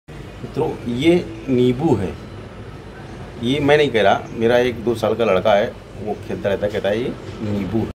अरे! यह क्या। आपने देखा क्या।
This is very useful fruit for health 👍👍👍
My baby calls it Neebu।